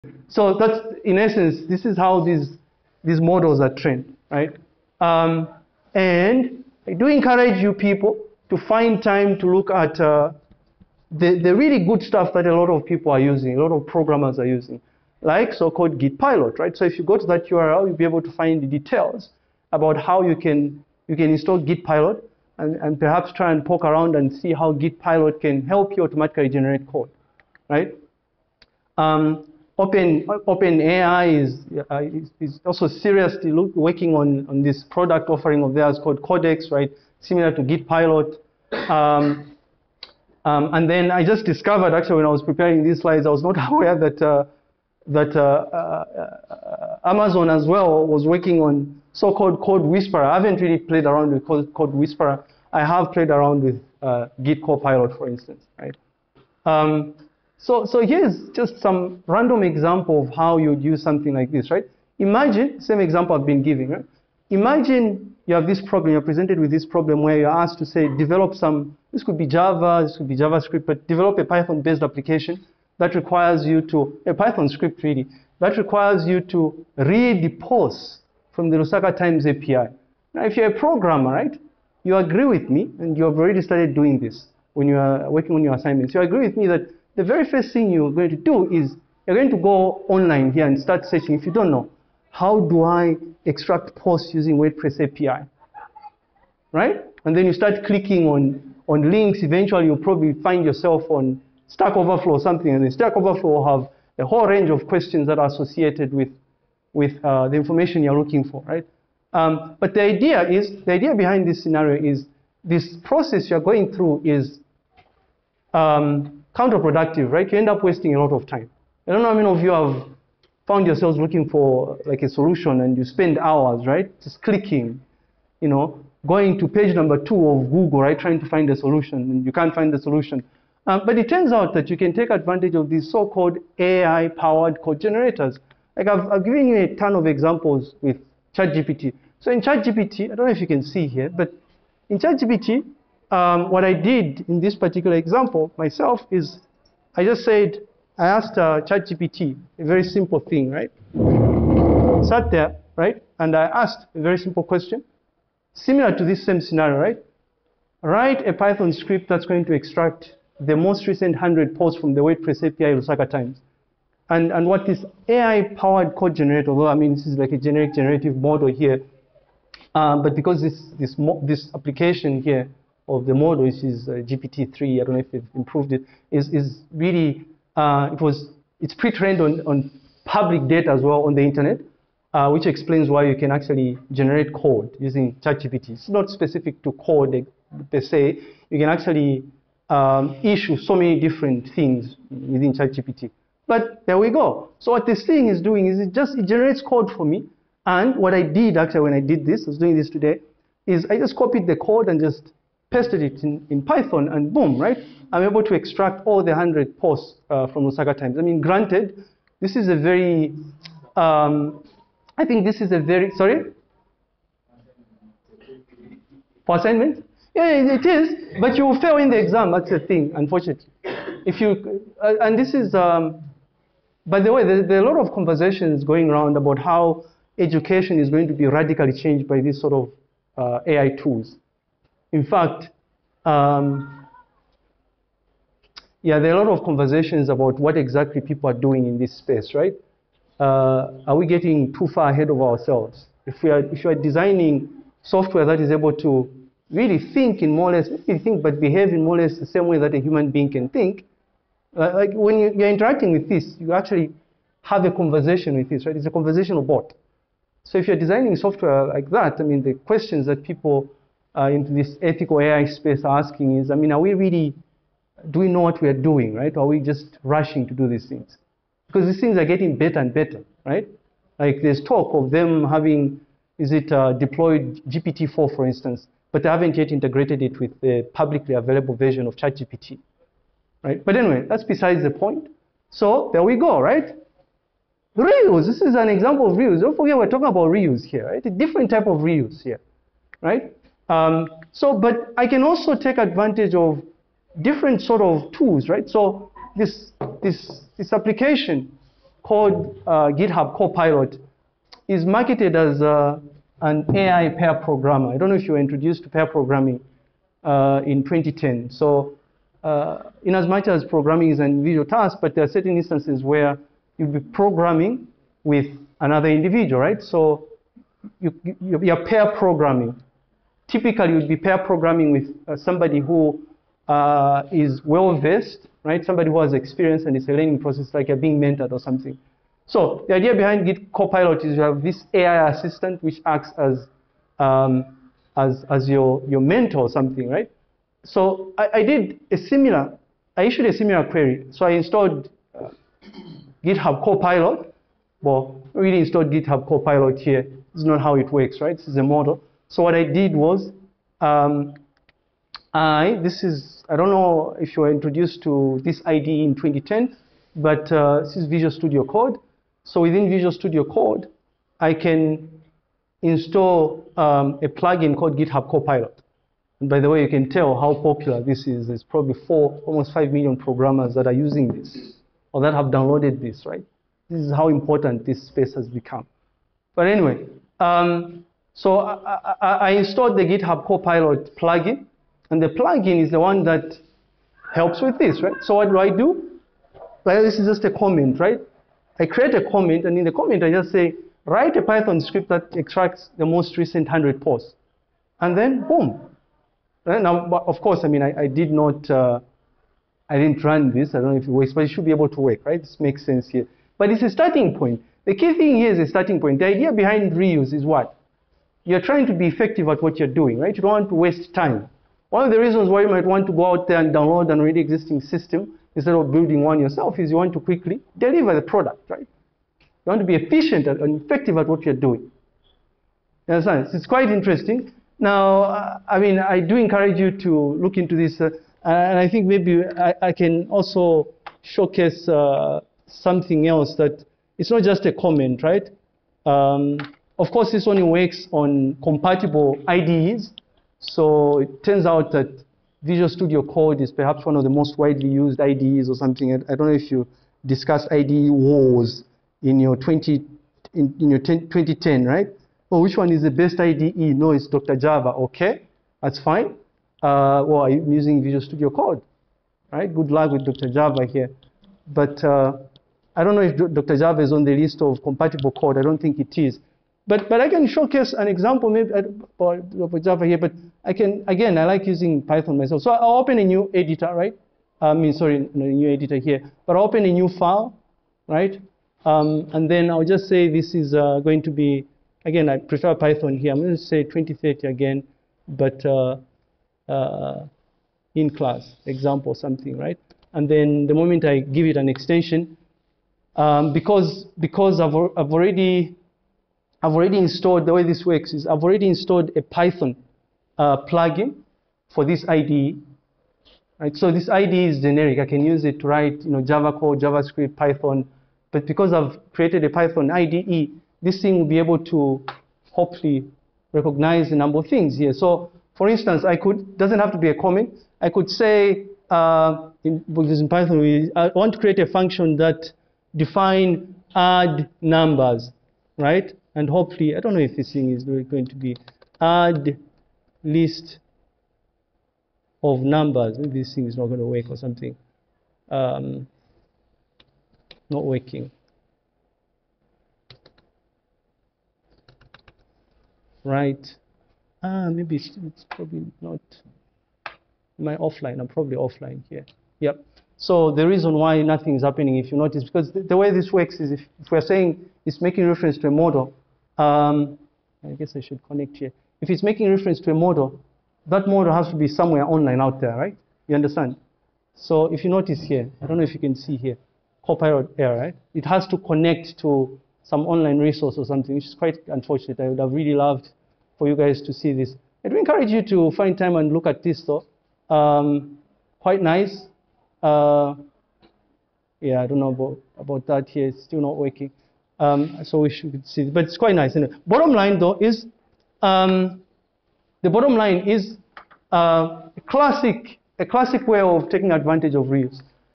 AI Code Generators: Reusable Software Components | Software Process Models | Software Engineering
Excerpt outlining software frameworks and re-usable software components.

Excerpt from Lecture Series #02: Software Process Models [1].